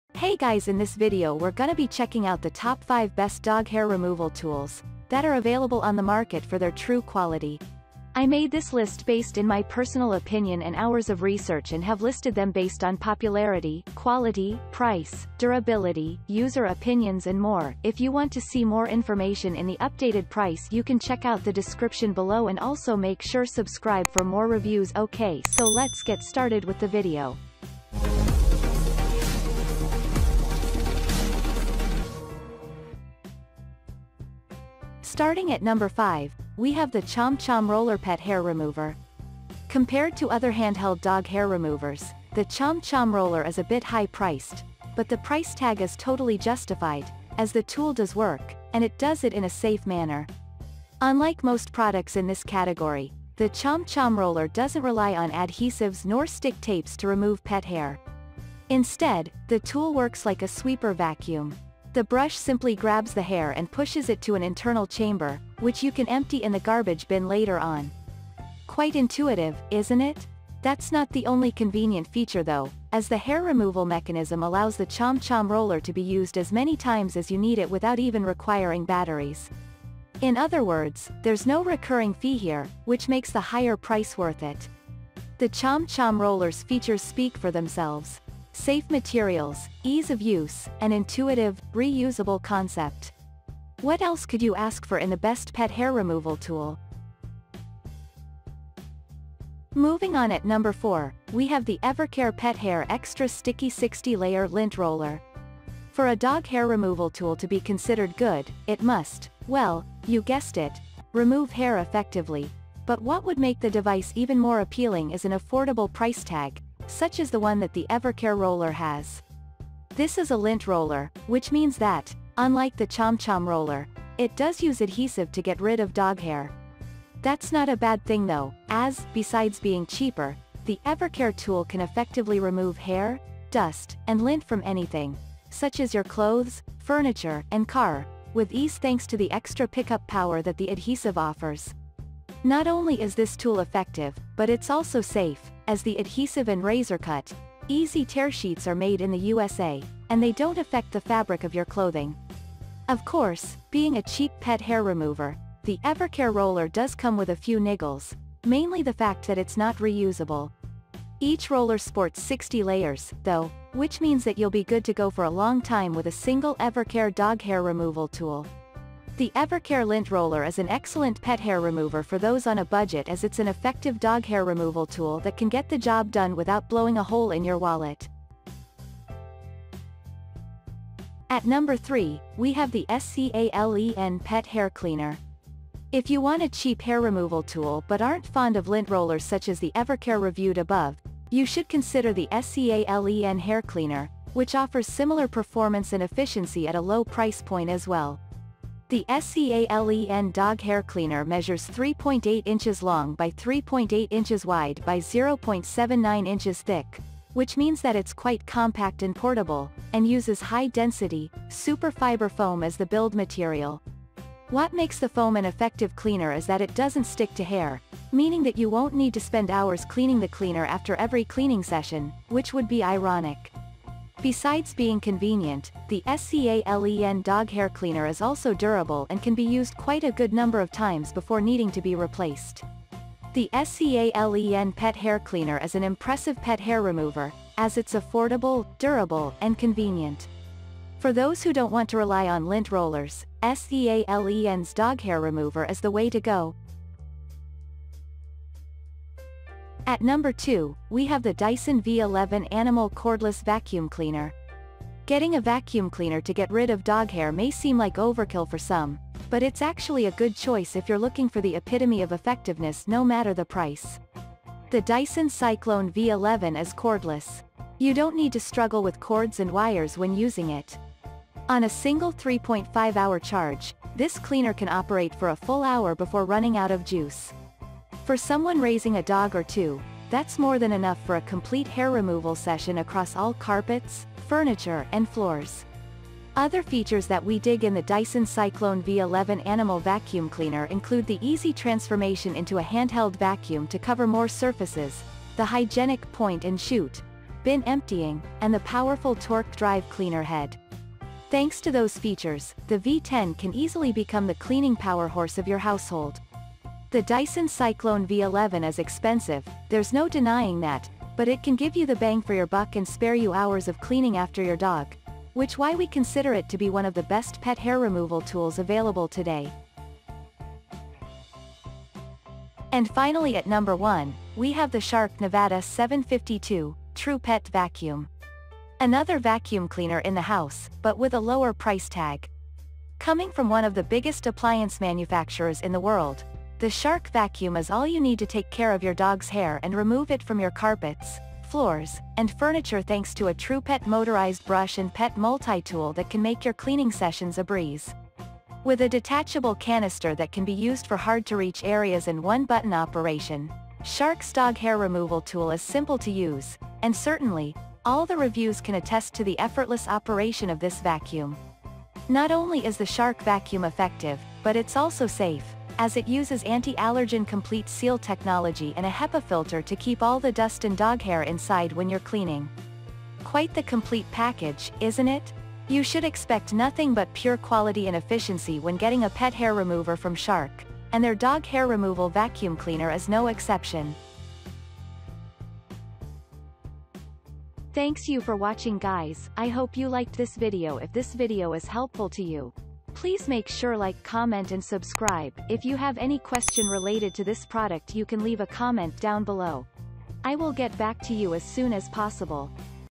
✅Top 5 Best Dog Hair Removal Tools of 2024
✅ 5. ChomChom Roller Pet Hair Remover

✅ 4. Evercare Pet Hair Extra Sticky 60 Layer Lint Roller

✅ 3. SEALEN Pet Hair Cleaner

✅ 2. Dyson V11 Animal Cordless Vacuum Cleaner

✅ 1. Shark NV752 TruePet Vacuum